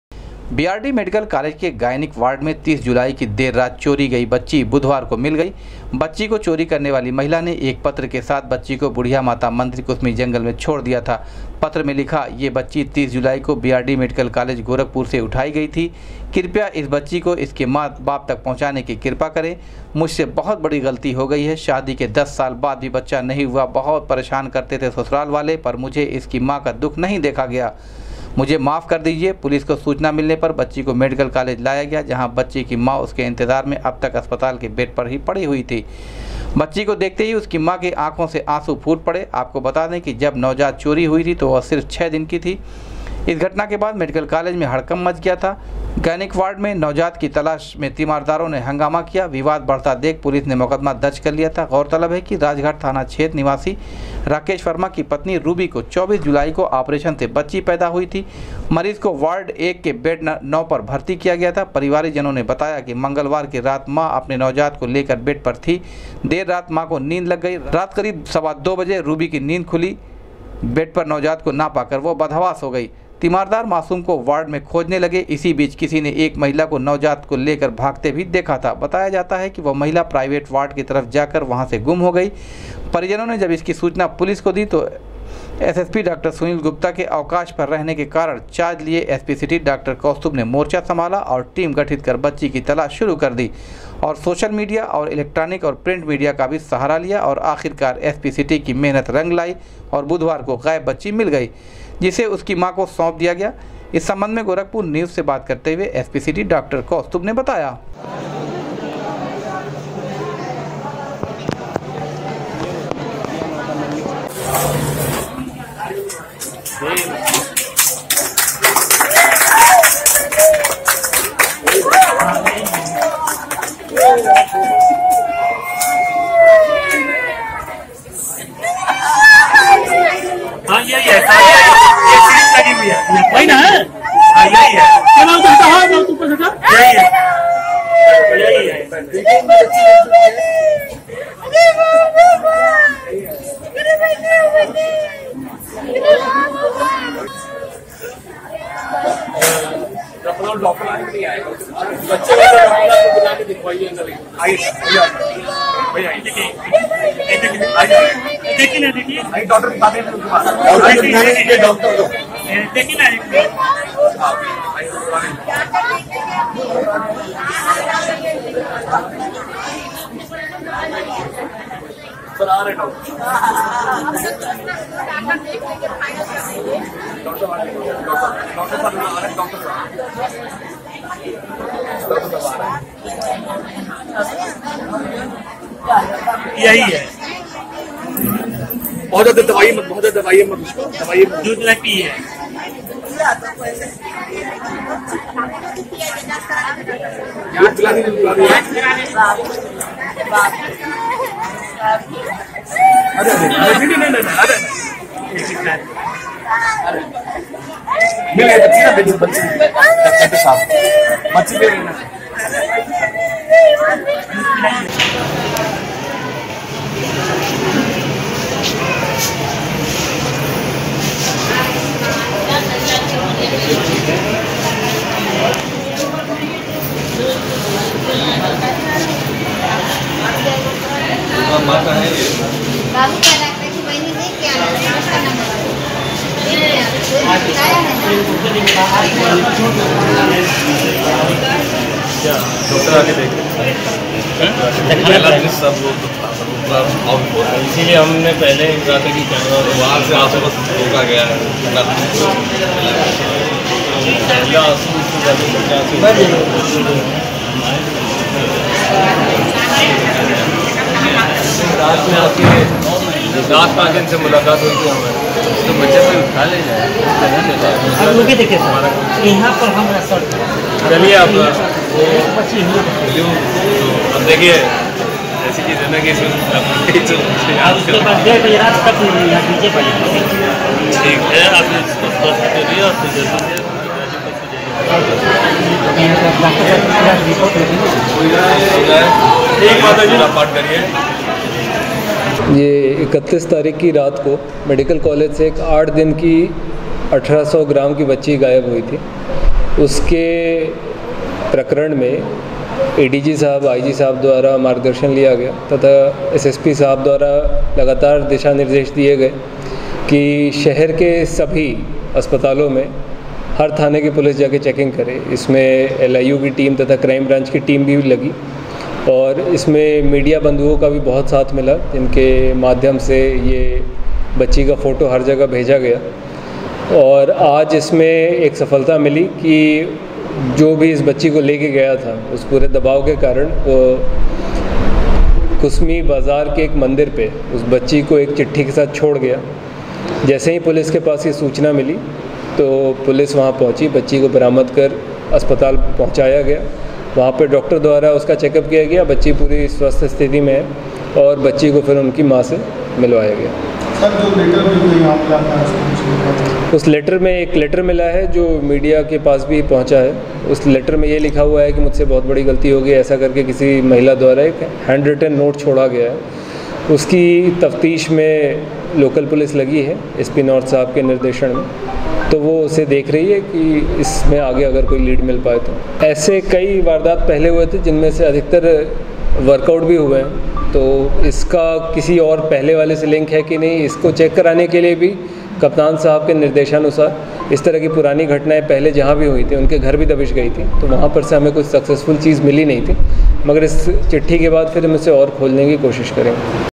एसपी सिटी की मेहनत लायी रंग, मेडिकल कालेज से गायब नवजात बच्ची हुई बरामद, मां की
एसपी सिटी की मेहनत लायी रंग, मेडिकल कालेज से गायब नवजात बच्ची हुई बरामद, मां की गोद में बच्ची आते ही मां का गम बदल गया खुशी में,परिजनों ने पुलिस को दिया धन्यवाद